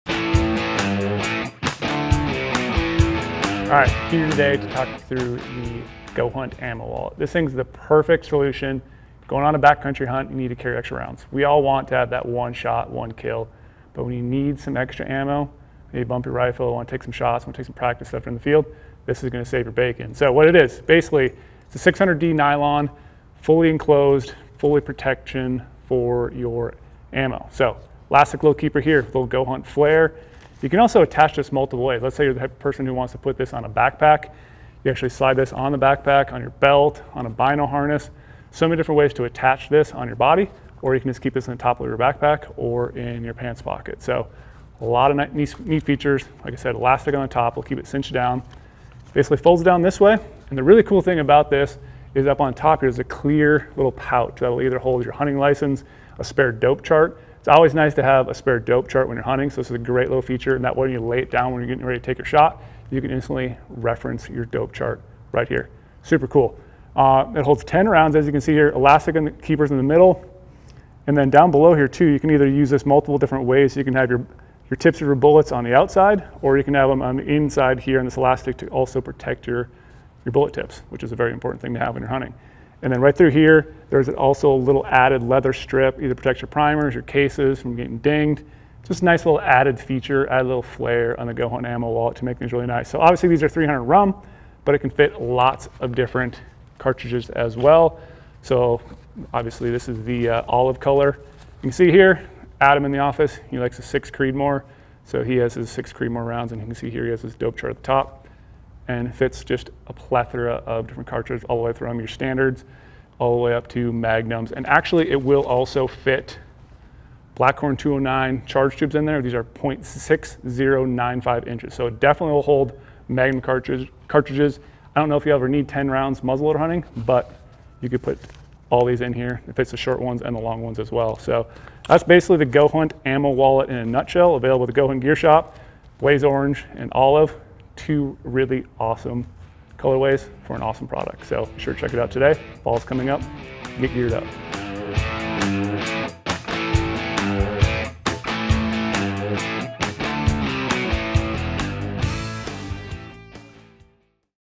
NEW GOHUNT Ammo Wallet
We have an amazing new ammo wallet designed here in house by our experts at GOHUNT. In this video Brady Miller goes over all the features of the new GOHUNT Ammo Wallet.